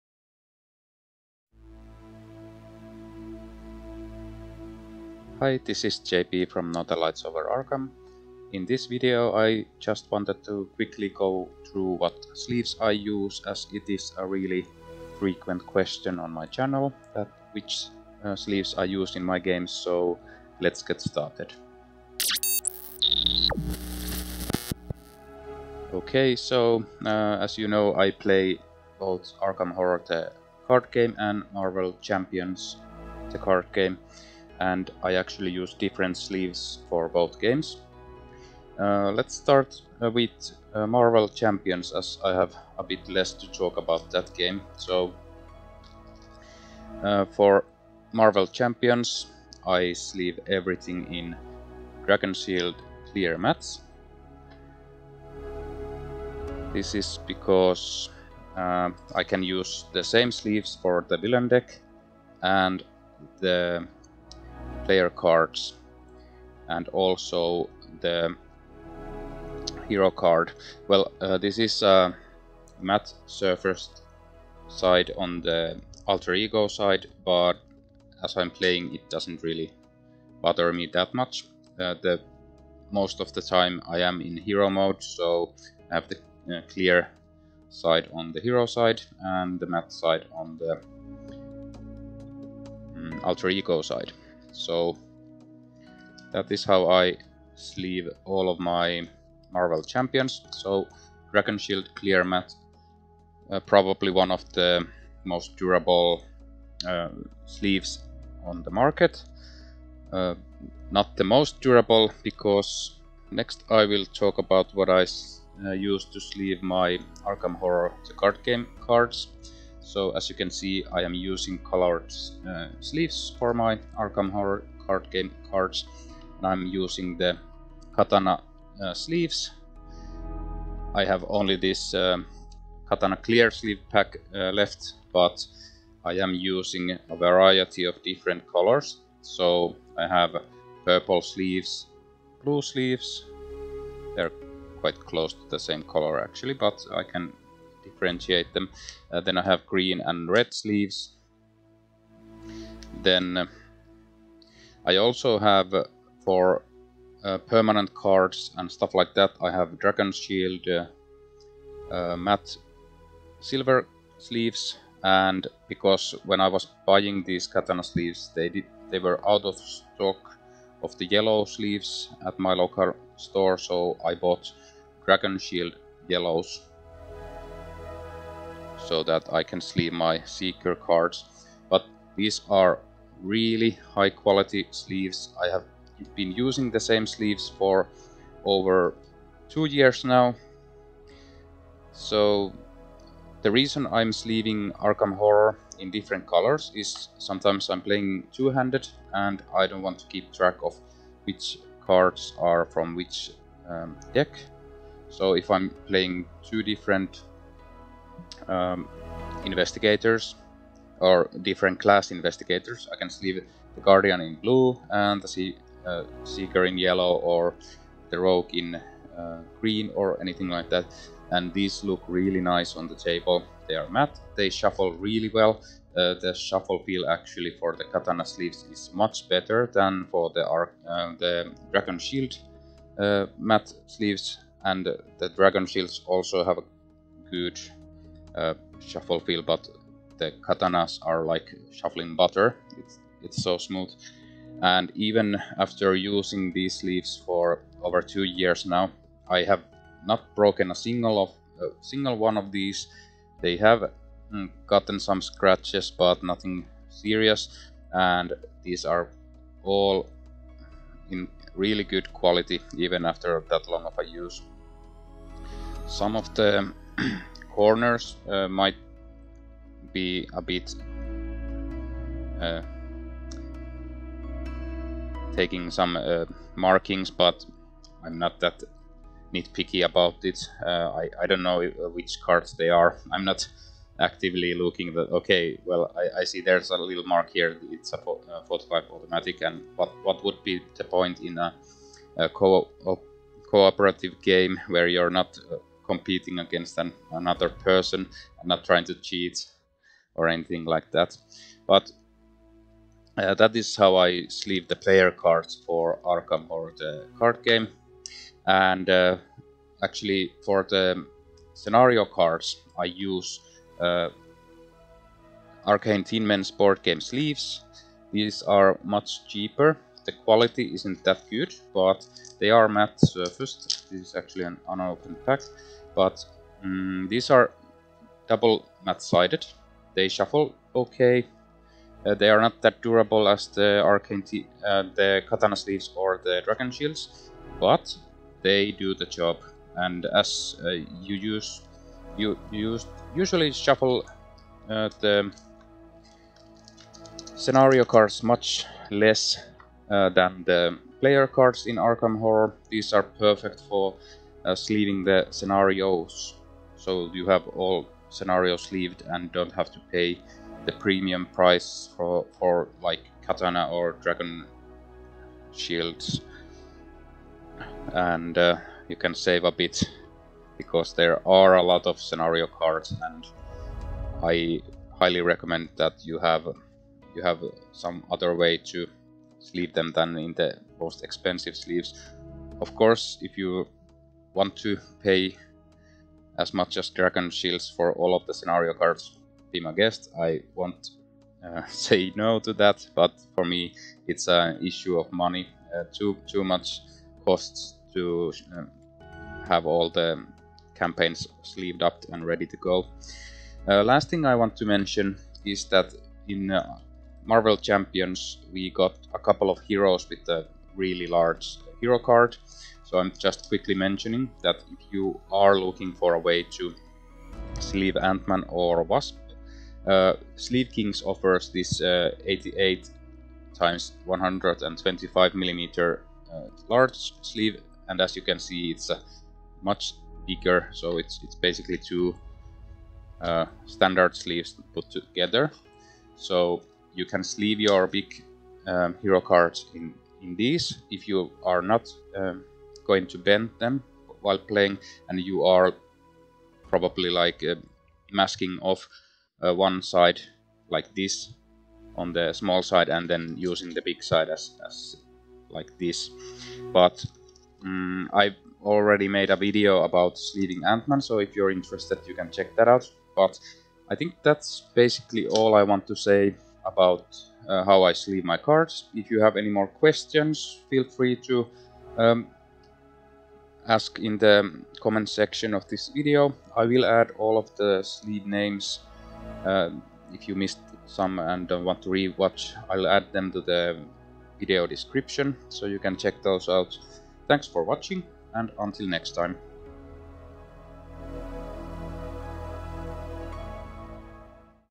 What Sleeves do I use for Arkham Horror and Marvel Champions LCG?
In this quick video I will go through all the different sleeves I use for Arkham Horror and Marvel Champions LCG. I get asked often what kind of sleeves I use in my videos, so I decided to make a quick video about it.

Sleeves I use:
Dragon Shield Clear Matte - Marvel Champions LCG
Dragon Shield Yellow Matte - Arkham Horror LCG
Ultimate Guard Katana Sleeves - Arkham Horror LCG (in red, blue, green and purple)
Arcane Tinmen Standard Size Matte Boardgame Sleeves - Arkham Horror LCG
Sleeve Kings extra large 88x125mm size sleeves - Marvel Champions LCG (Antman and Wasp Hero cards)